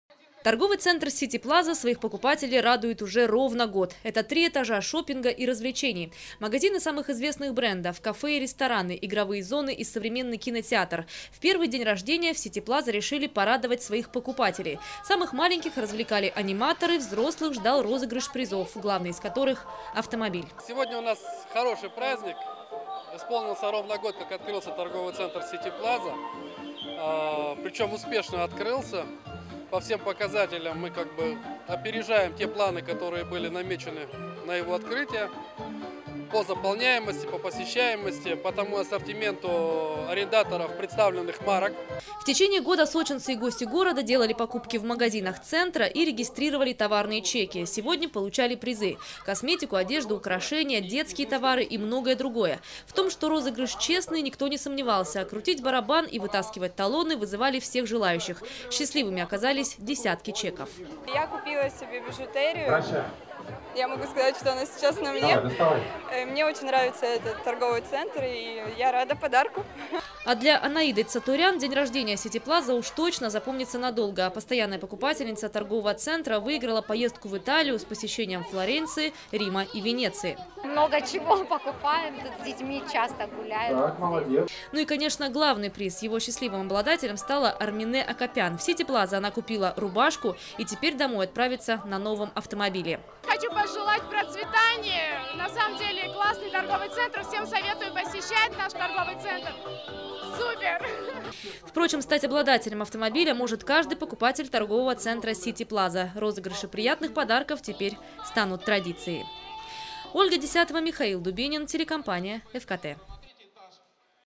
Сити Плаза в Сочи празднует день рождения Новости 24 Эфкате Сочи
Торговый центр City Plaza своих покупателей радует уже ровно год. Это три этажа шопинга и развлечений. Магазины самых известных брендов, кафе и рестораны, игровые зоны и современный кинотеатр. В первый день рождения в City Plaza решили порадовать своих покупателей. Самых маленьких развлекали аниматоры. Взрослых ждал розыгрыш призов. Главный из которых — автомобиль.

Сочи. Новости.
Телеканал Эфкате.